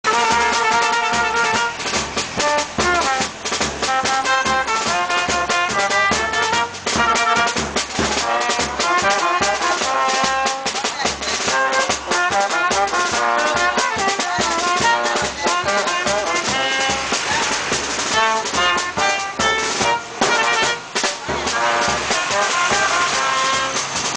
Animação com bandinha, Sombra, Chaplin e Minnie
Bandinha acústica com animação de personagens (Chaplin, O Sombra e Minnie).